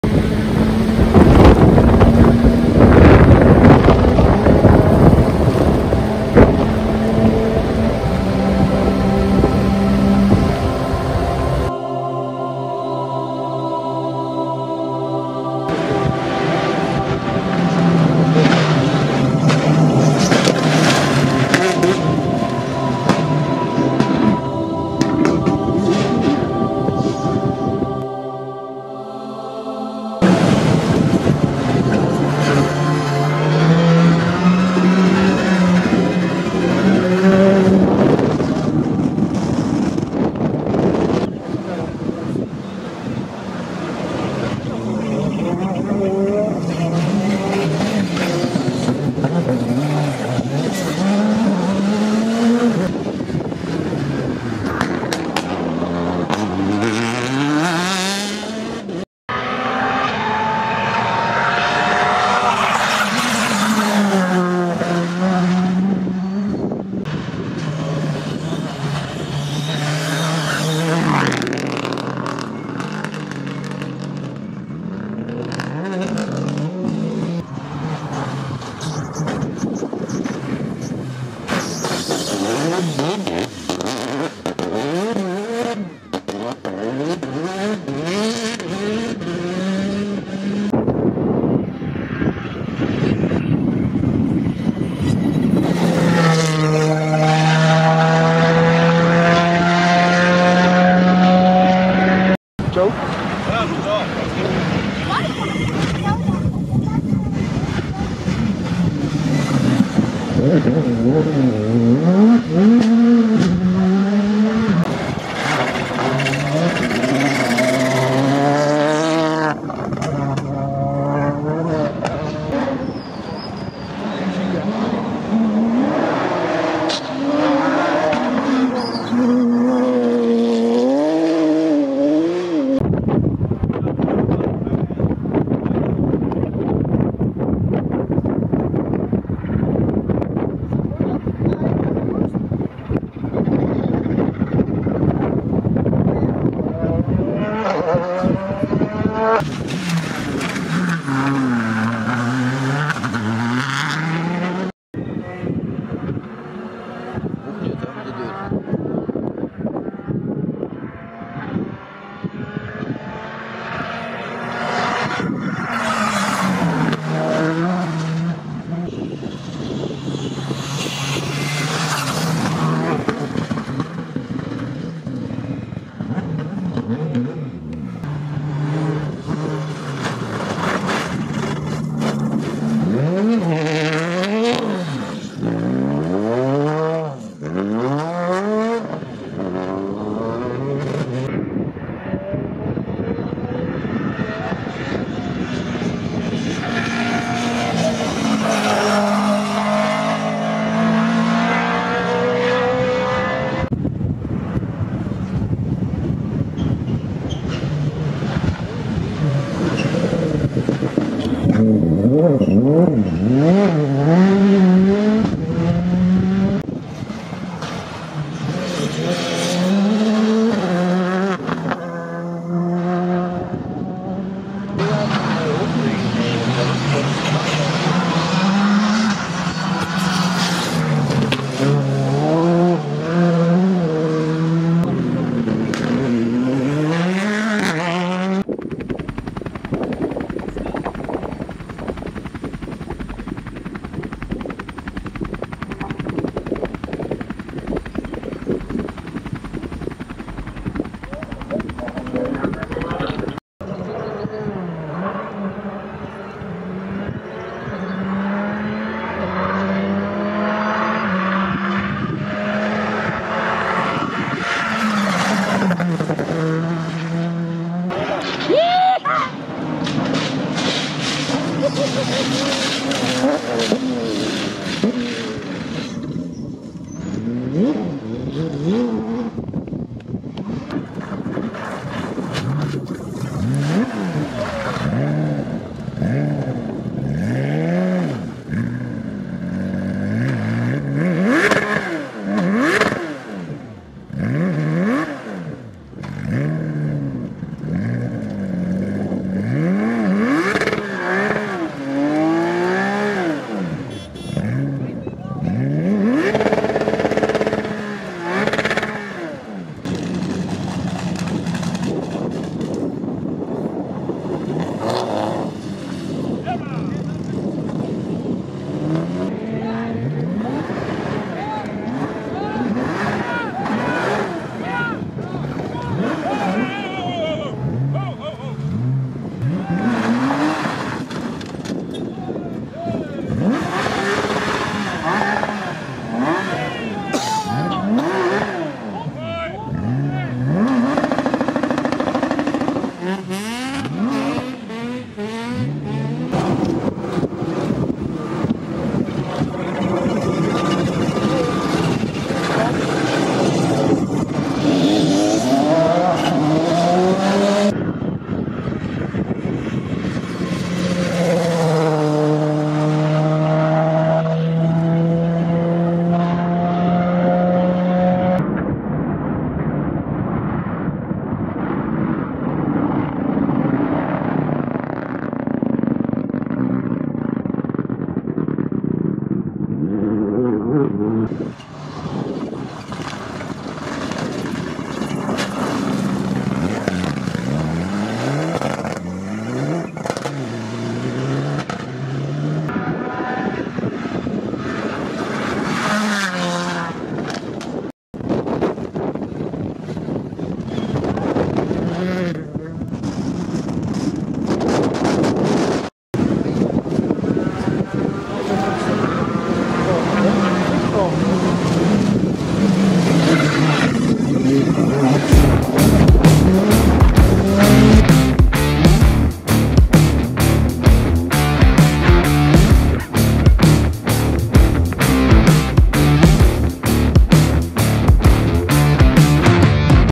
HASPENGOUWRALLY | 2020
Bende van Rally bezoekt de Haspengouw Rally 2020